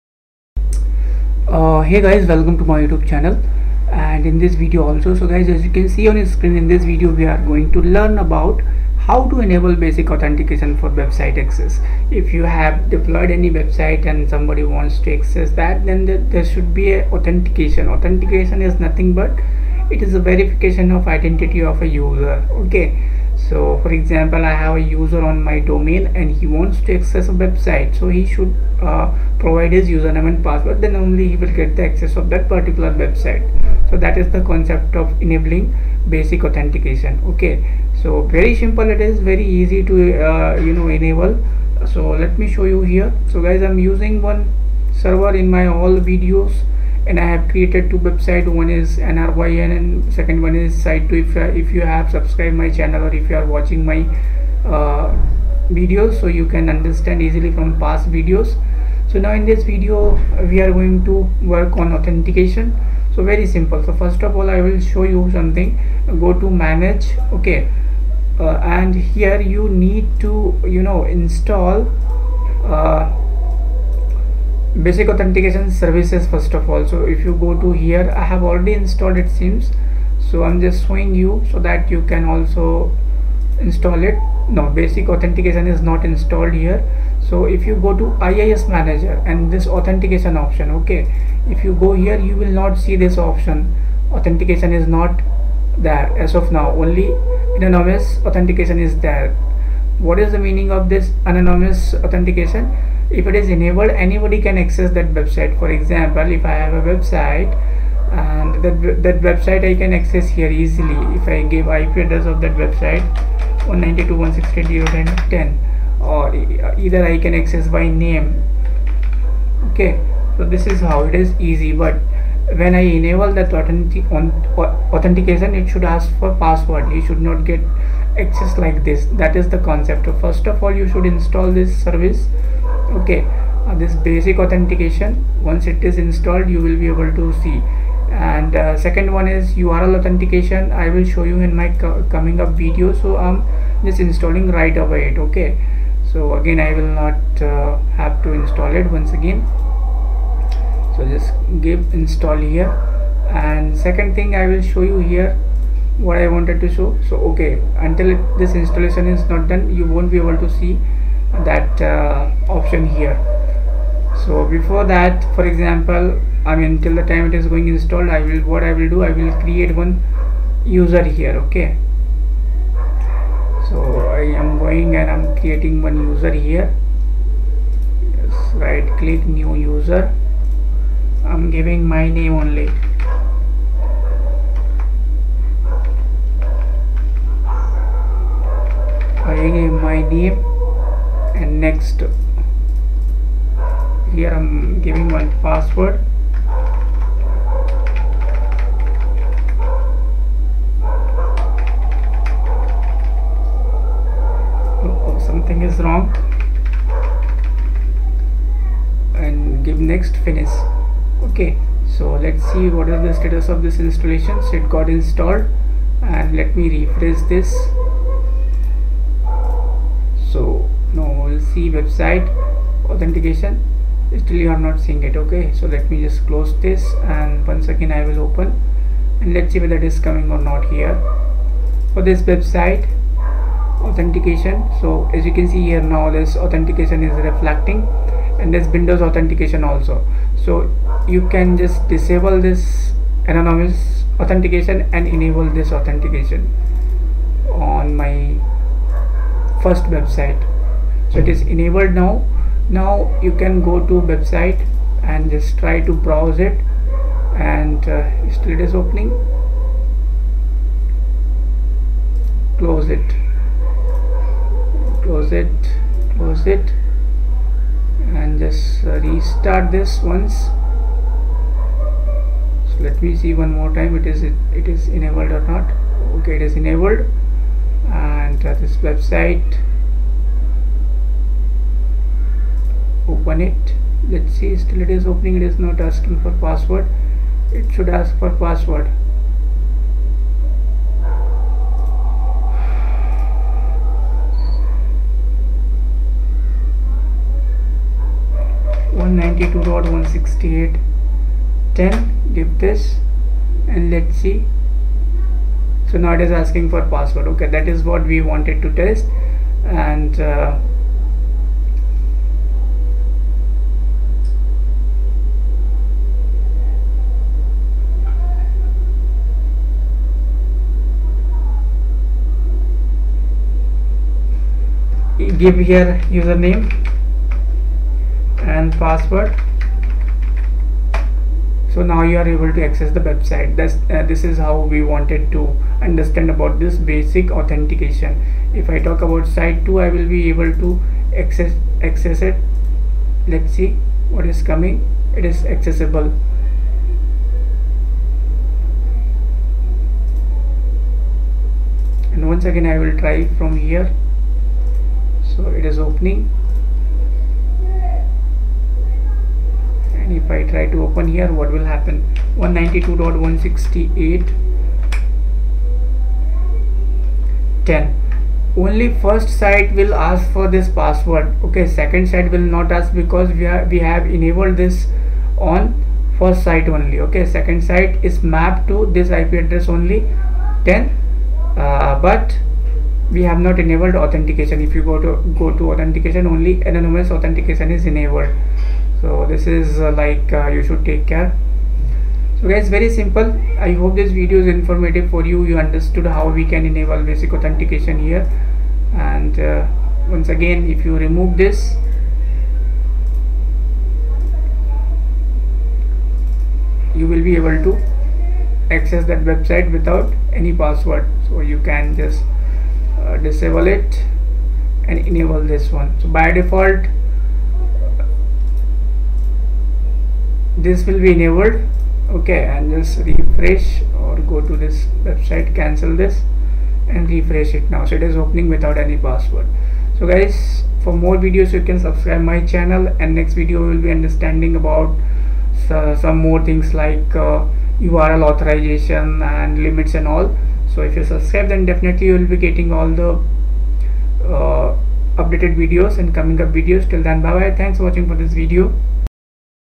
Web Server 2019 – IIS 10 How to Enable Basic Authentication For Web Site step by step - 09
Web Server 2019 – IIS 10 How to Enable Basic Authentication For Web Site step by step